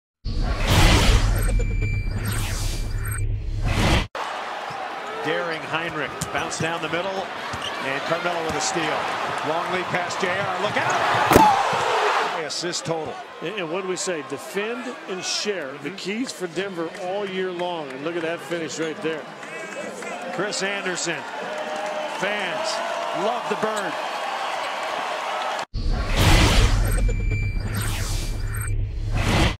J.R. Smith Statement Dunk
J.R. Smith catches the full court lob pass from Carmelo Anthony and finishes with a sweet dunk.